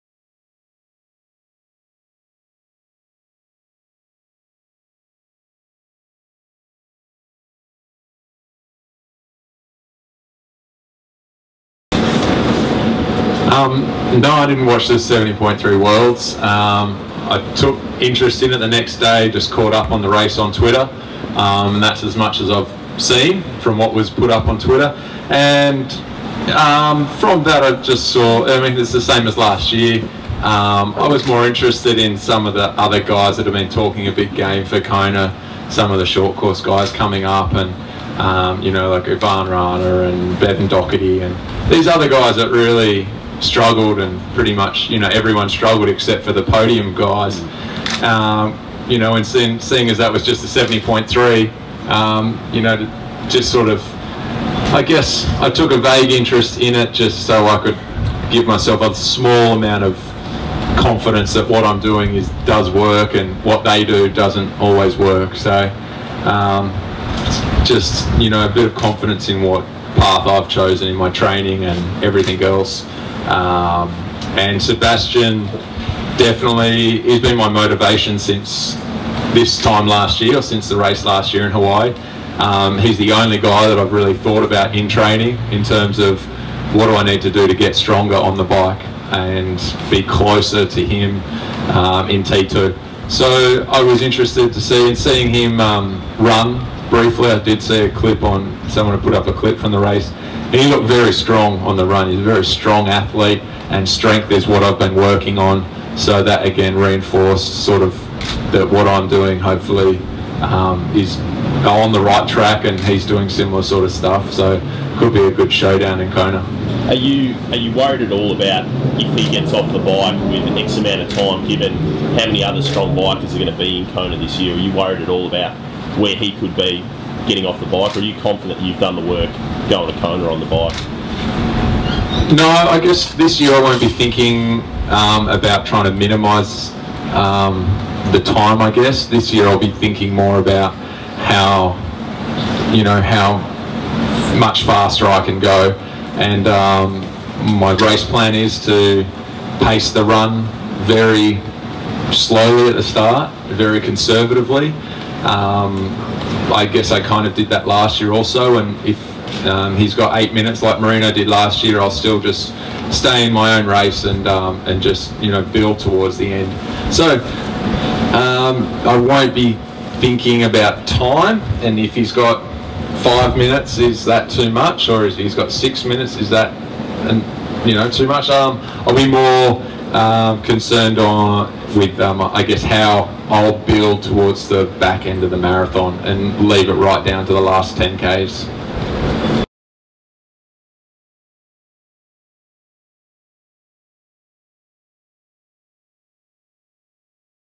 Pete Jacobs on facing Kienle in Kona
Pete Jacobs on racing Sebastian Kienle in Kona